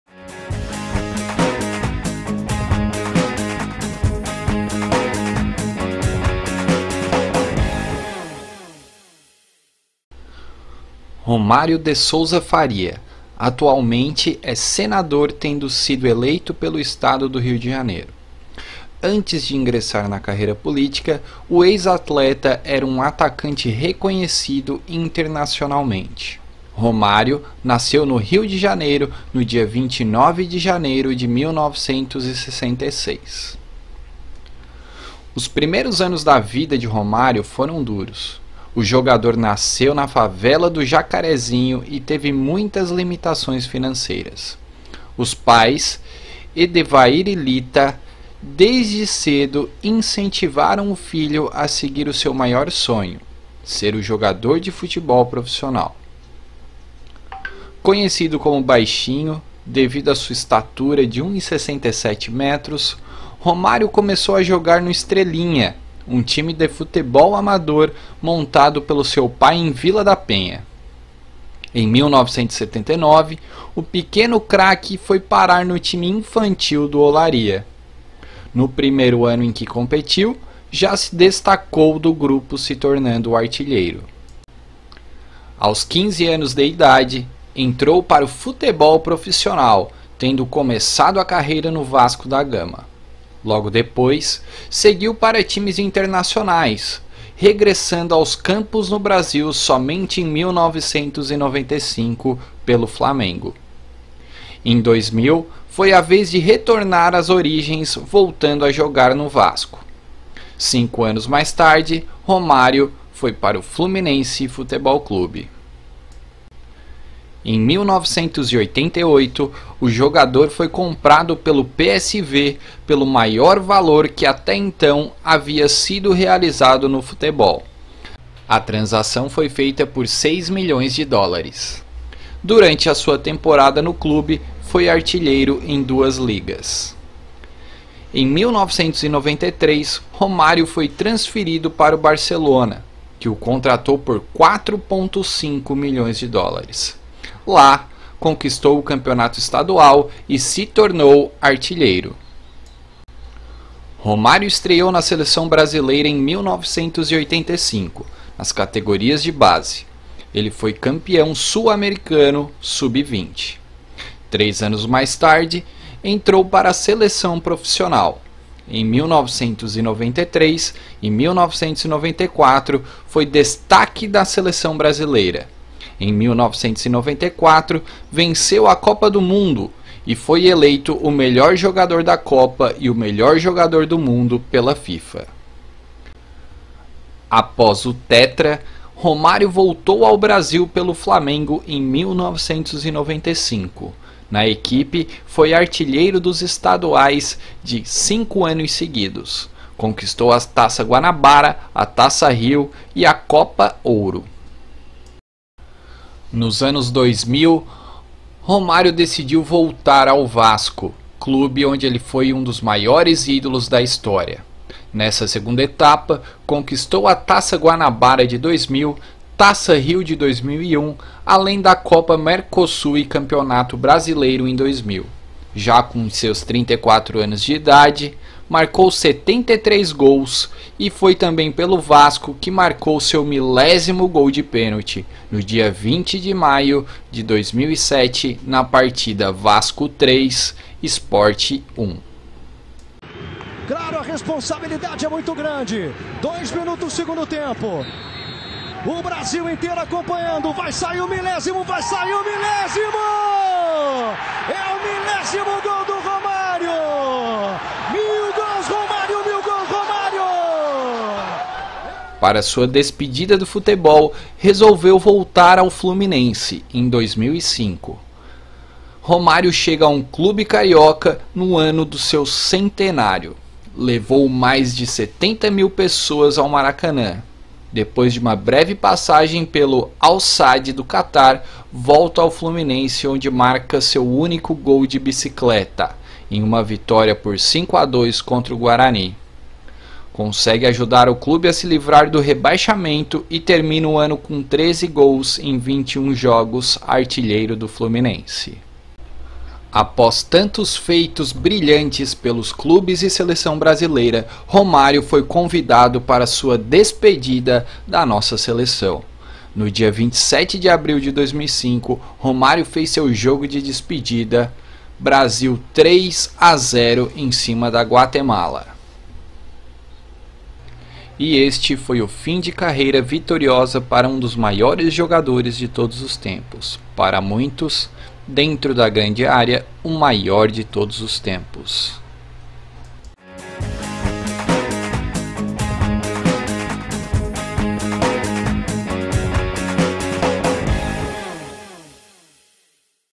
Tributo ao Romário - O REI DA GRANDE ÁREA
Para os apaixonados por futebol é impossível não conhecer e/ou admirar o legado futebolístico deixado por Romário de Souza Faria, conhecido como apenas Romário ou "Baixinho". Nascido no Rio de Janeiro em 29 de Janeiro de 1966, o carioca é filho do senhor Edevair de Souza Faria e de dona Manuela Ladislau Faria.

Morador do Jacarezinho durante a infância, Romário, apesar de muitos não imaginarem, sabia desde o princípio que o seu destino seria muito além dos campinhos de terra na Zona Norte da Cidade Maravilhosa. Idolatria, irreverência e muitos, repito, muitos gols resumem a vida de um verdadeiro gênio da grande área.

Este é o nosso tributo ao Romário! Espero que gostem!